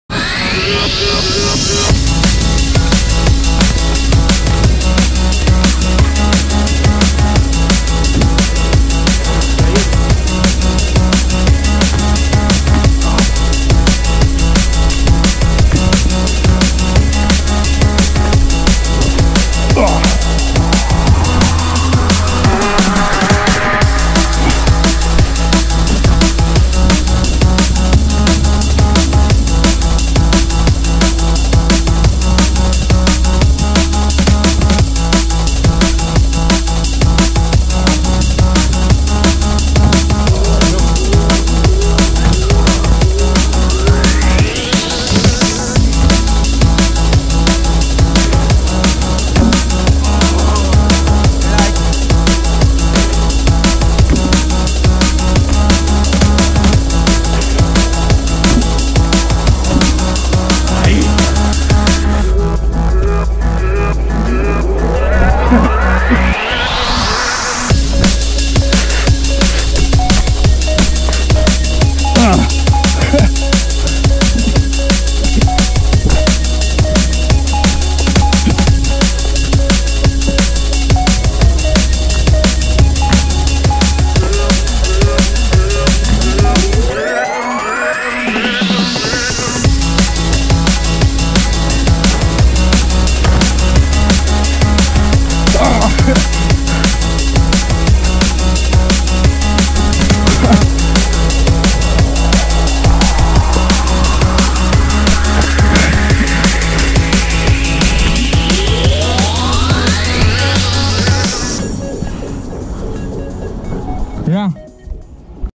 ES POSIBLE Entrenar PARKOUR en UNA HORA? (MIRA ESTE VIDEO)
(SI YA SE ES UN VIDEO VIEJO, LO TENIA GUARDADO) ;)

Sales de paseo y solo tienes una hora para entrenar! He aqui el resultado.
COMPARTAN Y NO OLVIDEN SUBSCRIBIRSE. GRACIAS!!